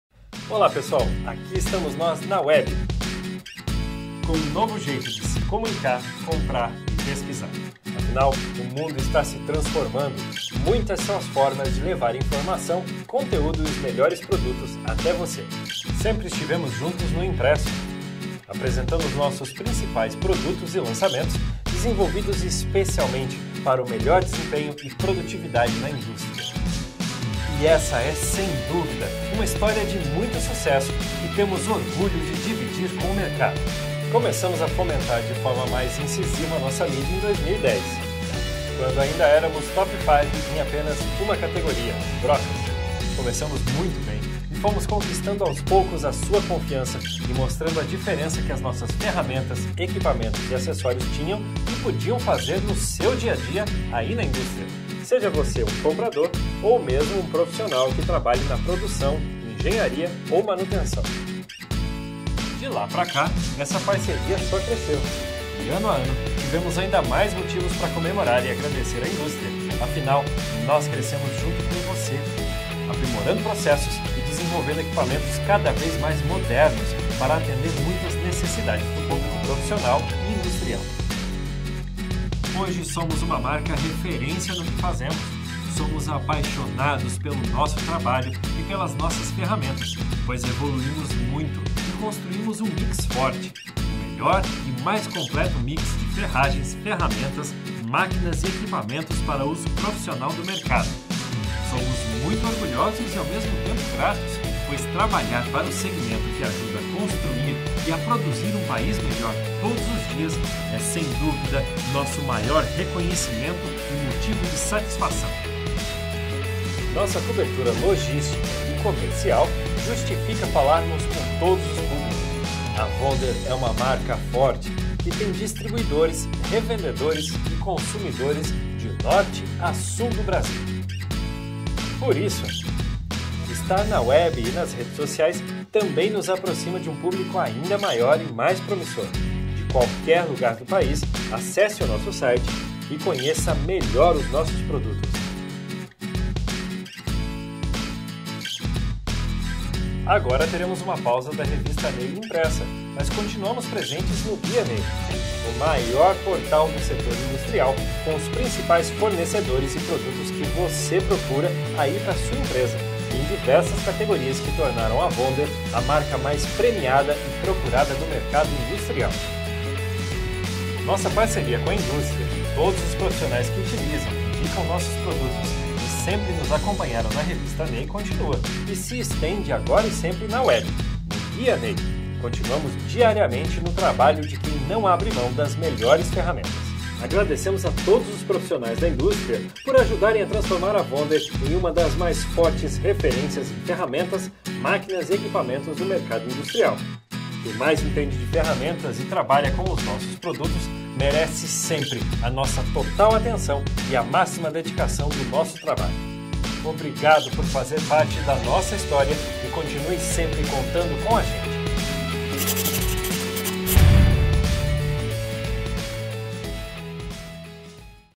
Agradecimento da VONDER ao mercado industrial!
Vídeo feito especialmente para a edição de novembro da Revista NEI com o nosso agradecimento à indústria e a todos os profissionais que nos seguem, escolhem, indicam, revendem e utilizam os nossos produtos.

Acesse também o nosso site, conheça nossos produtos e acompanhe todas as novidades pelo Facebook: